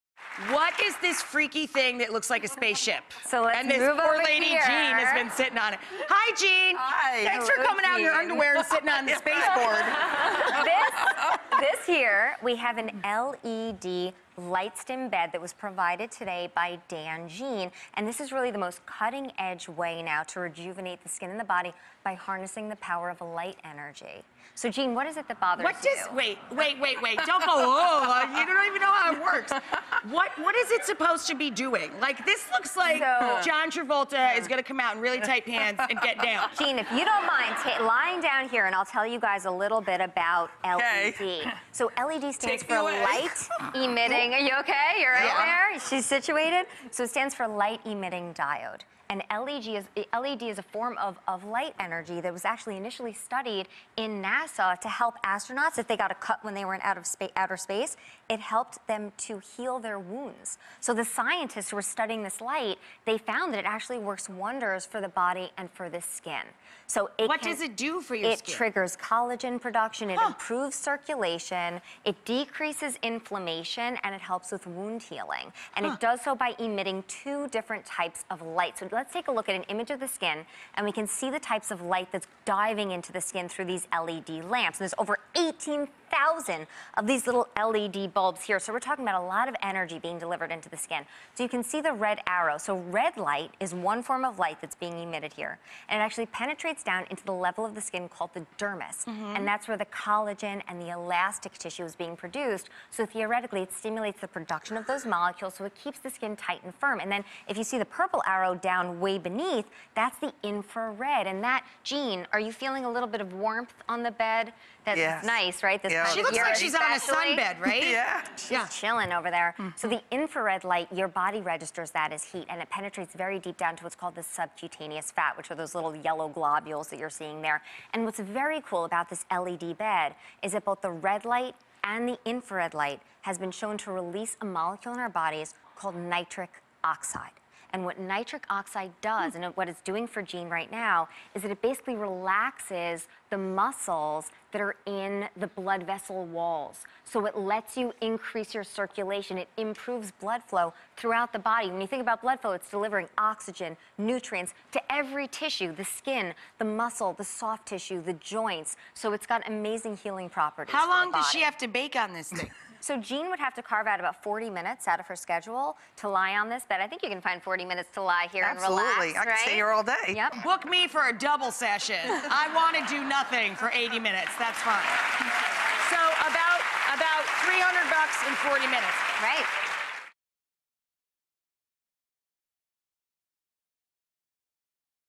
Can LED Light Improve Your Skin?
“This is really the most cutting-edge way to rejuvenate the skin by harnessing the power of light energy,” says Dr. Whitney Bowe of the futuristic-looking Lightstim Professional LED Bed by Dangene